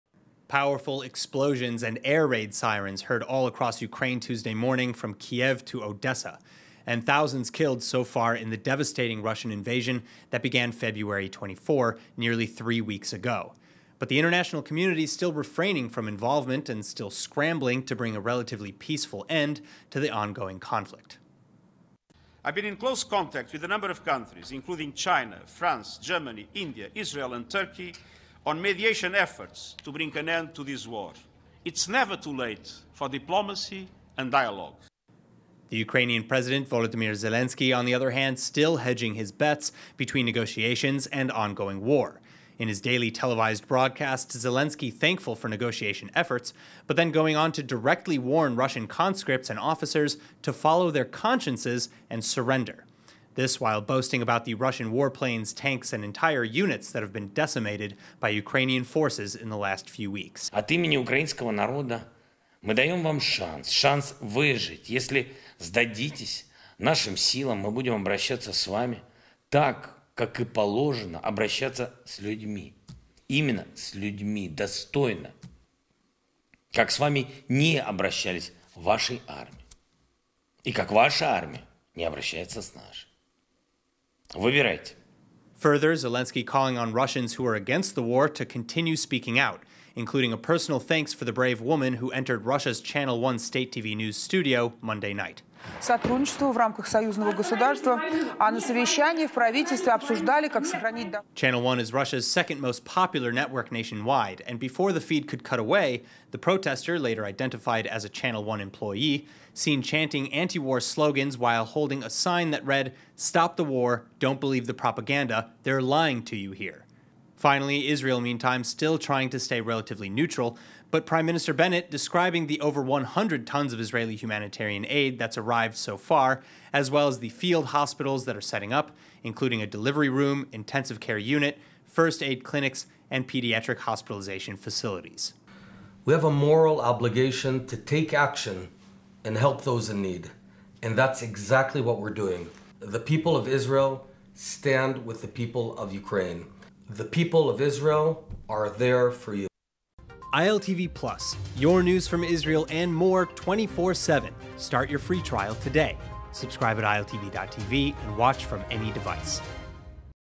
Ukraine President calls on Russian forces to surrender
The Russian invasion entering day twenty, today… with the Ukrainian capital of Kyiv still under heavy fire, Tuesday morning. And this, after the latest rounds of talks, Monday evening, ending with no progress again.